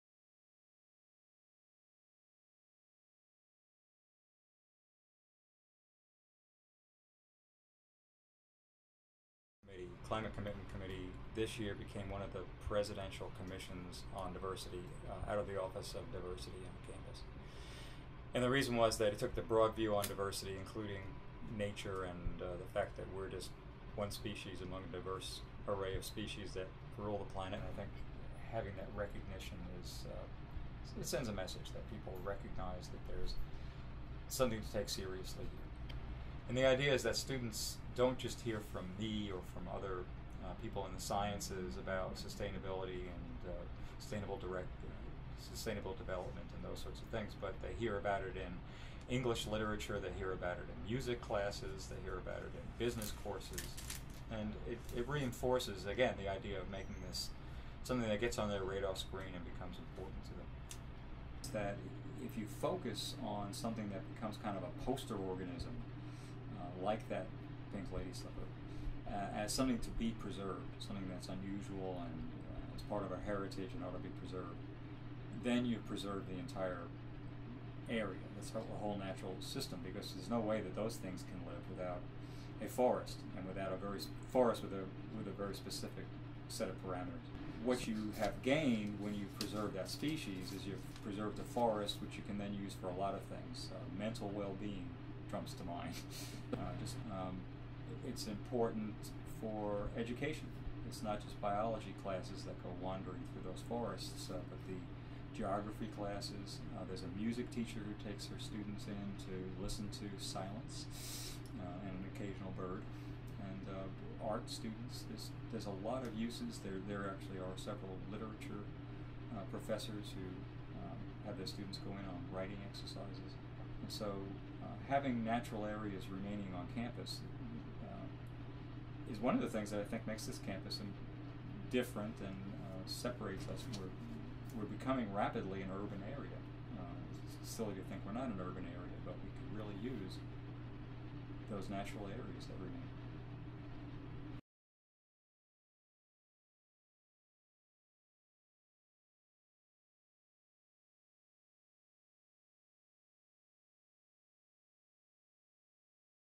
RC Paul: "Father of Sustainability"
KENNESAW, Ga. - RC Paul, director of sustainability at Kennesaw State University, was honored as one of the recipients of the inaugural Presidential Diversity Awards. The Award for Distinction in the Sustainability category recognized Paul's decades-long efforts to preserve natural areas on campus, promote energy efficiency and grow the University's recycling programs. He was a driving force behind the creation of Kennesaw State's Climate Commitment Council and its observance of Earth Hour and Earth Day.